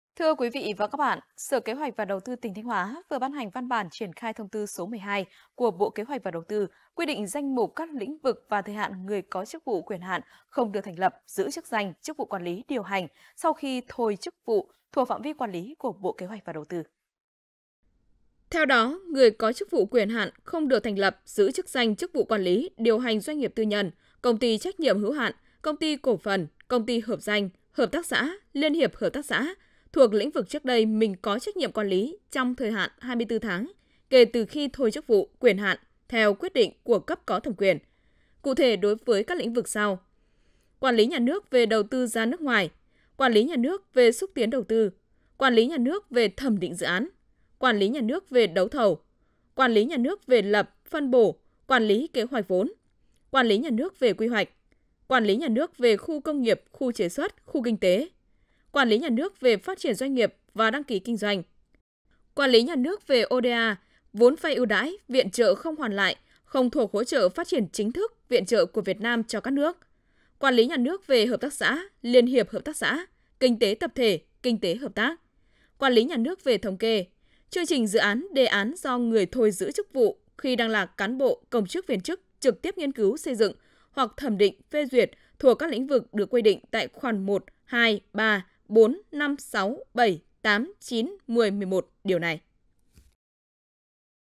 Danh mục các lĩnh vực và thời hạn người có chức vụ quyền hạn không được điều hành sau khi thôi chức
Sở Kế hoạch và Đầu tư tỉnh Thanh Hoá vừa ban hành văn bản triển khai Thông tư số 12 của Bộ Kế hoạch & Đầu tư quy định danh mục các lĩnh vực và thời hạn người có chức vụ, quyền hạn không được thành lập, giữ chức danh, chức vụ quản lý, điều hành sau khi thôi chức vụ thuộc phạm vi quản lý của Bộ Kế hoạch và Đầu tư.